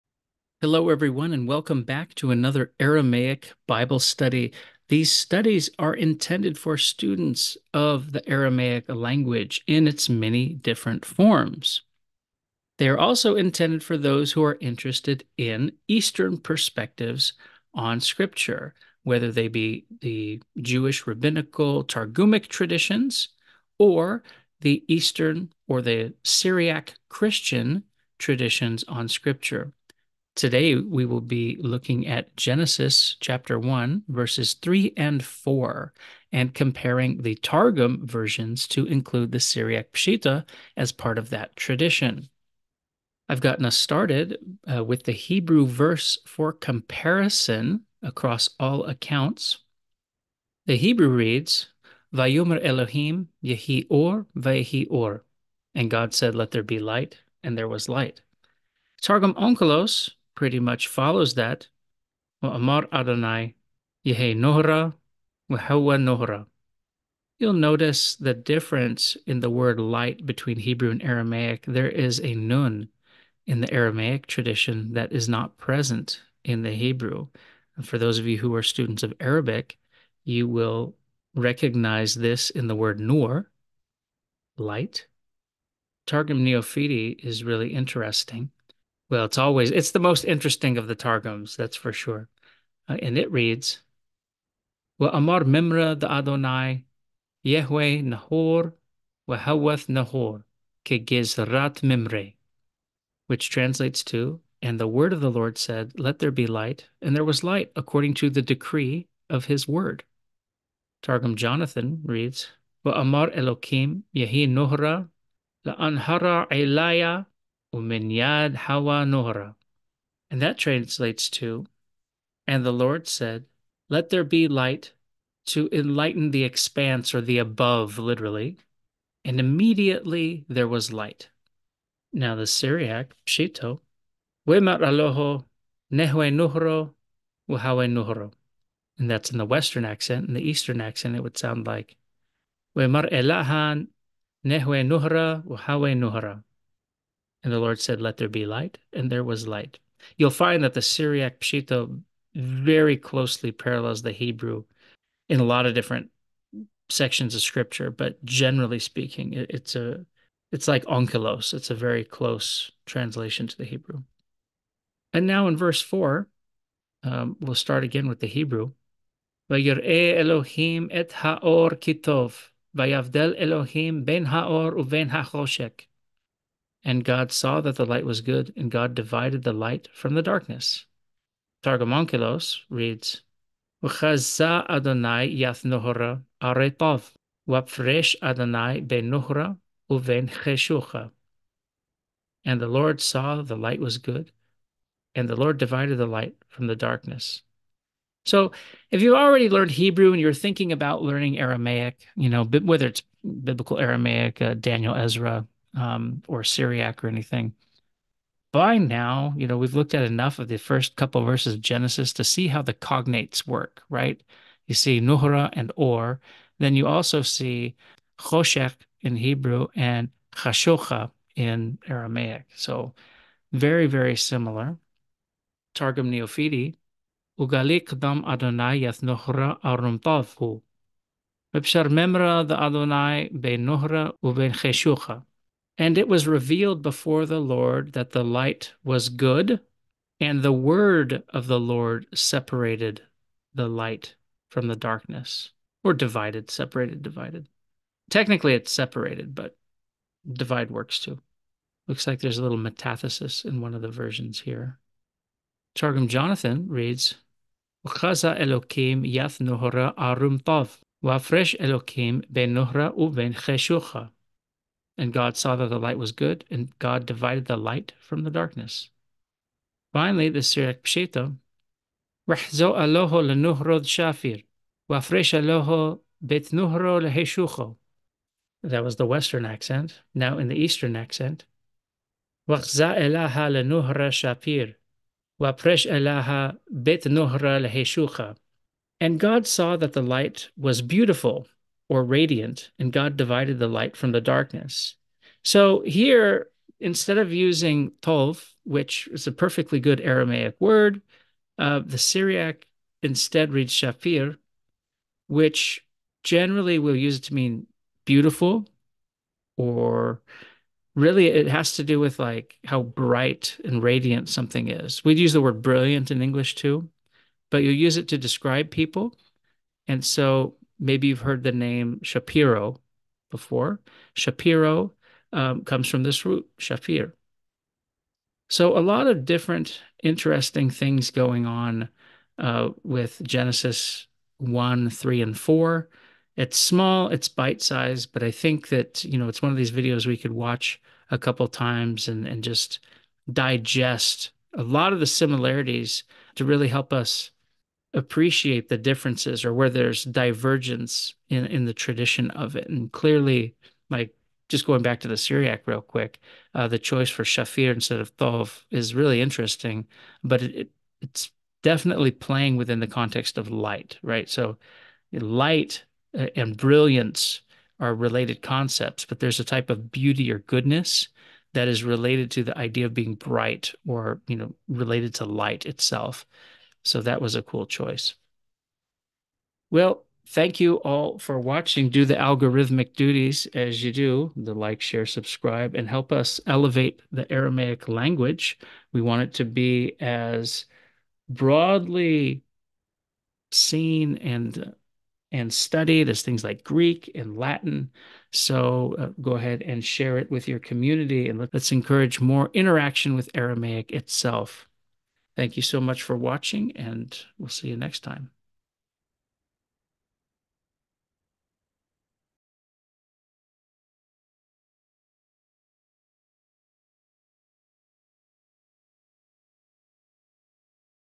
Let There Be Light. Was the light "good" or "beautiful"? Aramaic Bible Study Genesis 1:3-4
Can the Aramaic Targum traditions help us better understand the Hebrew the creation of light found in the book of Genesis?

The following is from my Aramaic Bible Study series, covering the third and fourth lines of the Bible, Genesis 1:3-4, in the various Aramaic Targum traditions, including the Syriac Peshitta. How can the Aramaic versions of Genesis 1:3-4 better inform our understanding of the verse?

Students of Bible Aramaic should naturally progress to reading the Aramaic Targums of the Old Testament, though few get the opportunity to do so. I hope by examining the Aramaic Targums, former students of Biblical Aramaic can reignite their Aramaic skills and develop new insights by reading texts that they would normally only read in Hebrew.